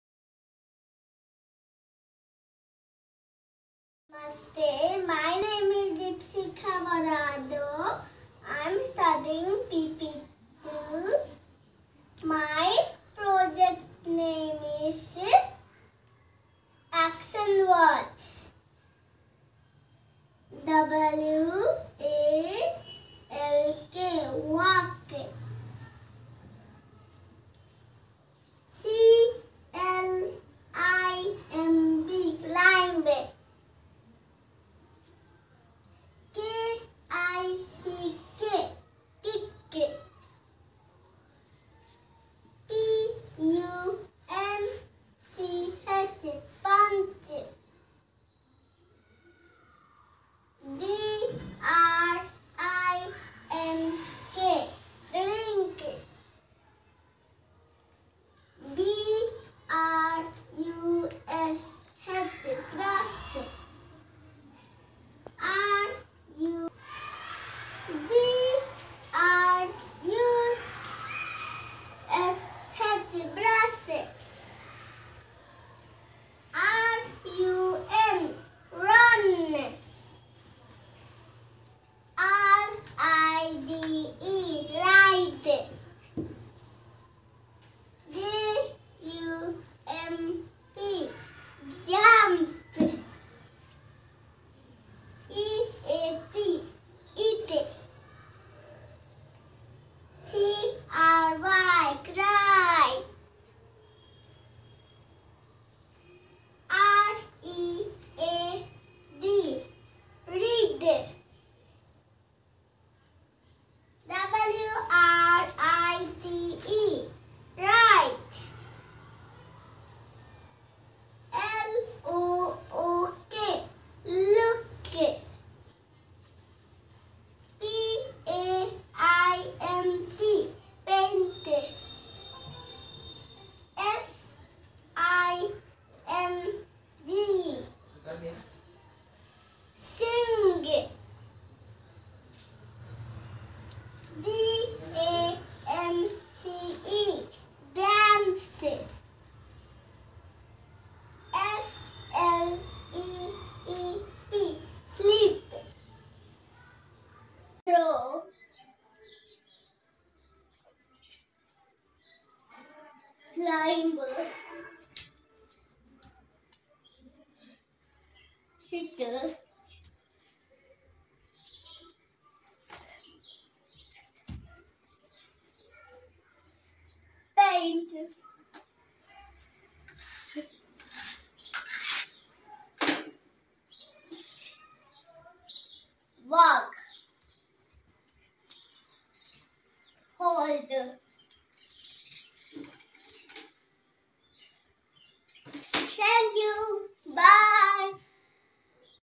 Action Words 🏃🧎‍♀️🚶‍♀️🕺🧍‍♀️
Walk, stamp, jump, run, skip, hop, crawl, bend .

Sway, swing, shake, twist, gallop, leap, roll .

Kick, tip-toe, twirl, grab, punch, pull, push .

Wiggle, catch, throw, dig, wave, climb, wink .

Clap, yawn, blink, shuffle, creep, march, turn ...

Ride, swim, dive, skate, dance, jog, stomp.

These are the actions we all can do.